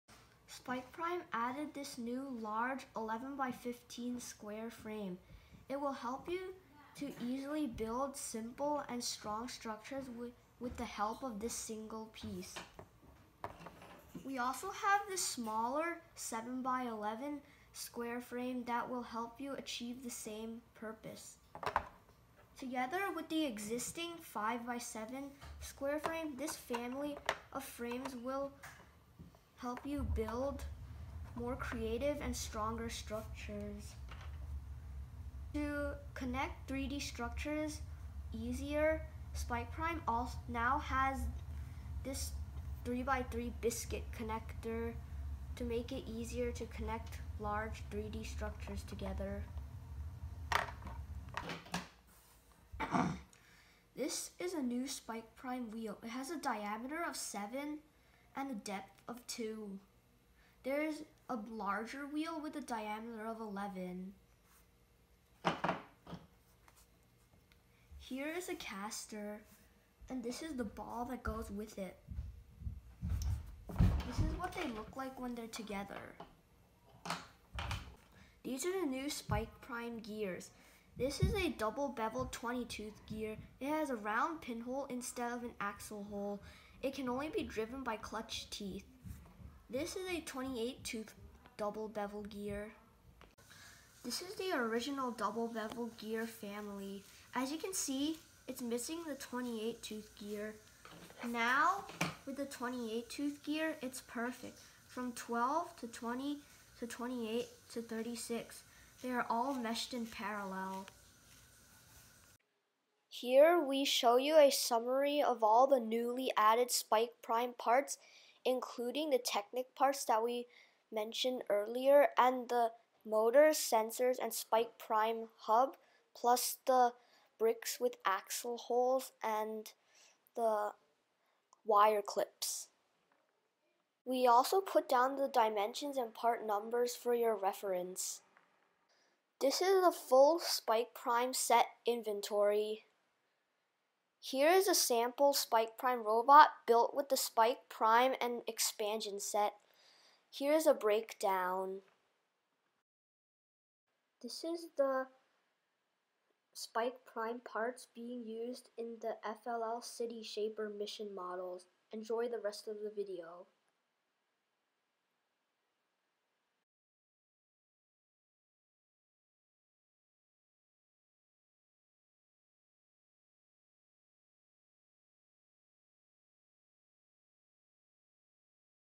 List of New Parts from LEGO Spike Prime
We show you the inventory of all newly created spike prime parts and its usage in FLL City Shaper mission models.